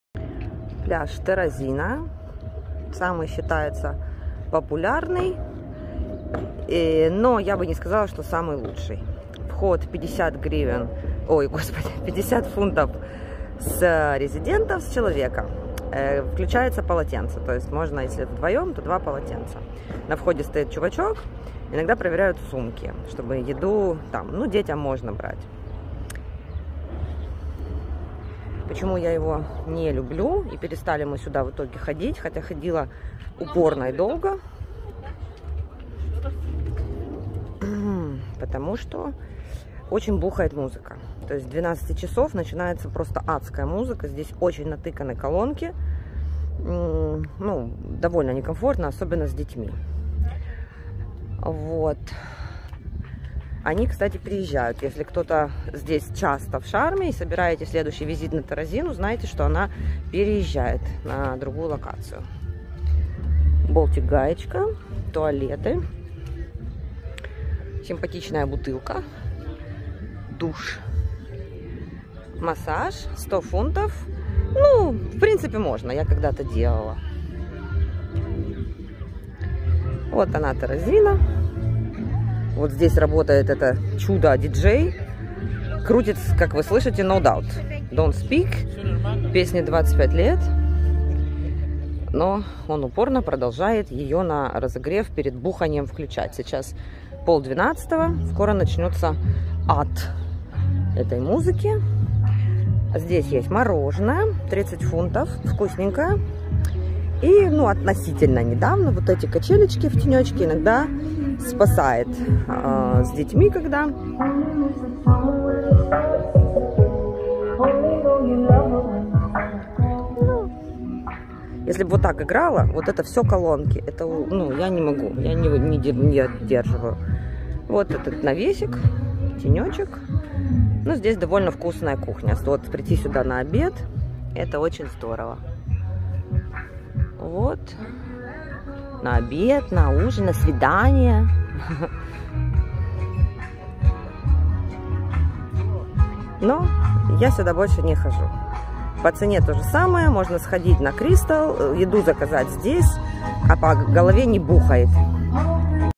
Самый популярный пляж TERAZZINA | Шарм Эль Шейх [Египет]
Обзор самого популярного пляжа в Шарм Эль Шейхе - TERAZZINA. Один из лучших пляжей Египта. Плюсы: развитая инфраструктура, доступная цена, чистый пляж, вкусная кухня. Минусы: громкая музыка.

Здесь про Шарм эль Шейх, жизнь, туризм, интересные места и события.

Буду рада пообщаться, пишите комментарии или найдите меня в соц.сетях указанных ниже.